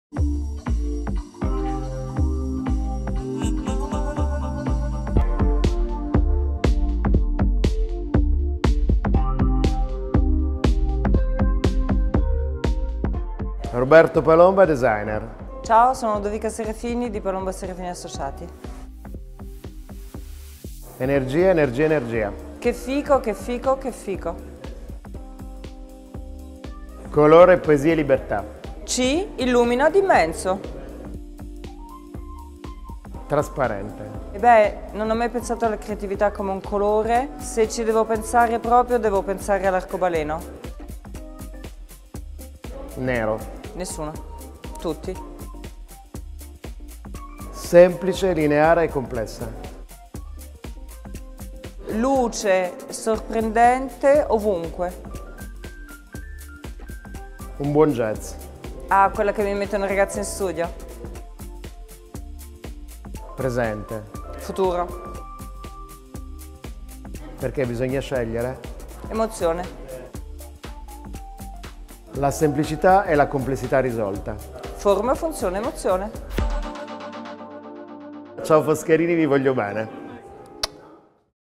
Foscarini at MDW 2019 | Interview with Roberto+Ludovica Palomba
Ready for Euroluce 2019!
We have chosen this exhibit design to narrate who we are: an incubator of ideas. Foscarini is a company that gathers, develops and activates multiple design ideas and creative contributions. At Euroluce 2019 Foscarini presents 10 new projects, from Marc Sadler, Andrea Anastasio, Ferruccio Laviani, Ludovica & Roberto Palomba, Ilaria Marelli and Tord Boontje.

Listen to the interview with Roberto + Ludovica Palomba presenting their new projects and their personal view about design, Milan Design Week and Foscarini.